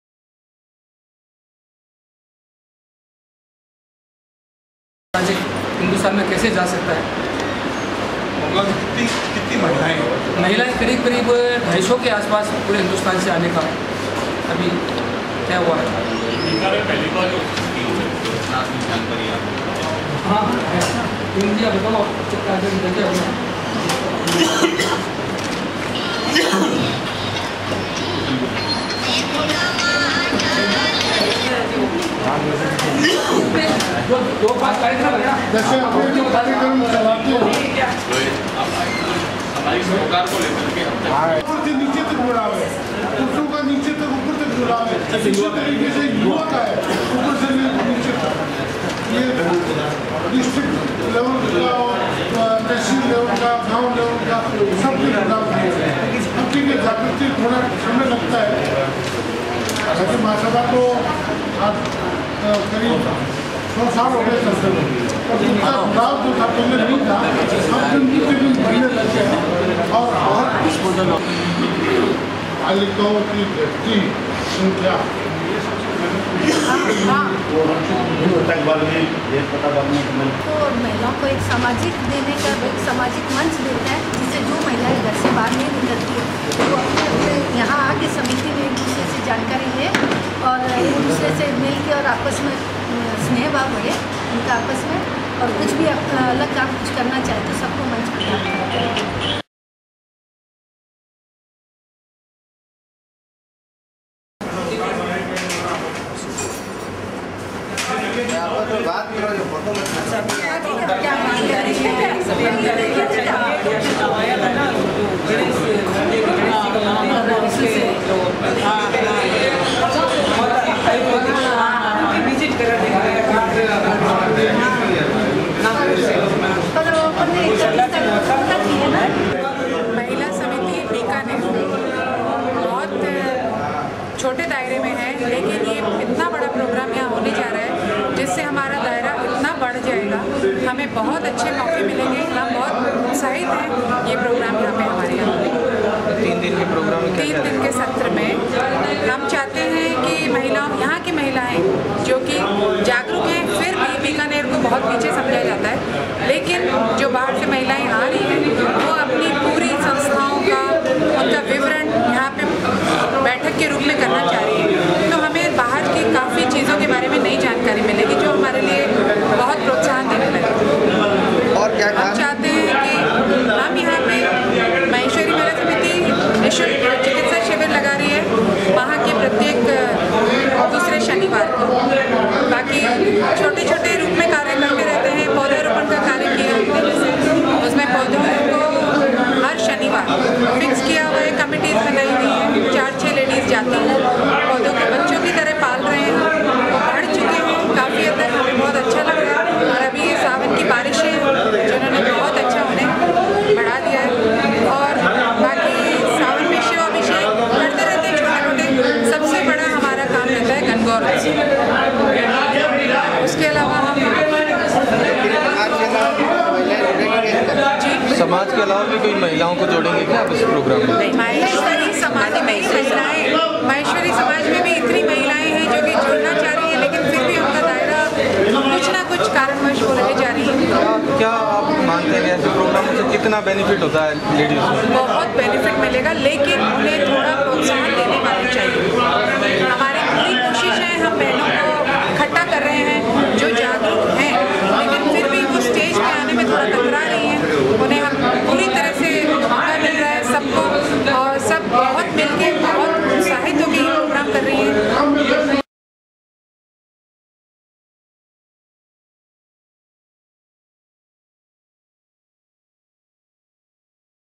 All India Maheshwari Women Organisation
अखिल भारतवर्षीय माहेश्वरी महिला संगठन का त्रि दिवसीय कार्यक्रम एक अगस्त से Read full story at khabarexpress.com